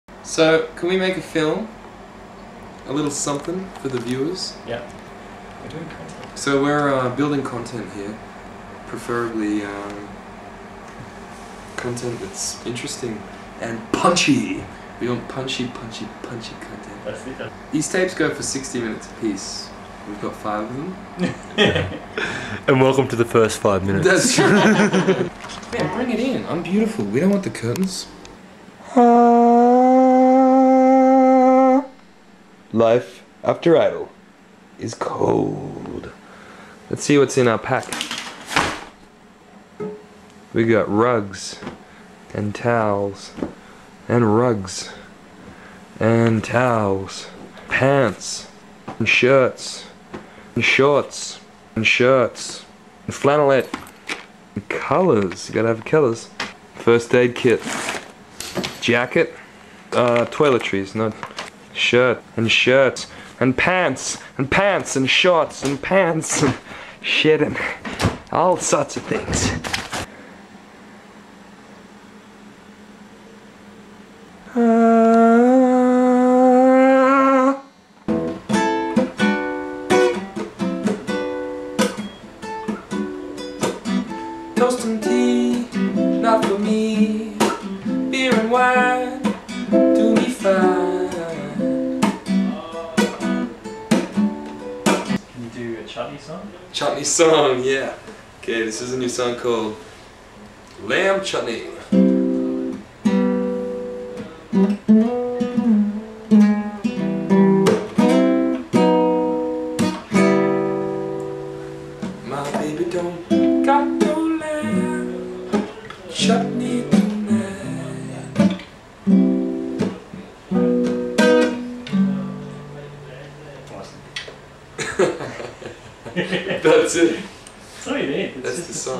Bobby Flynn Tour Diary 01
We're at Armidale! Bobby has been making himself at home, playing random songs and unpacking his bag.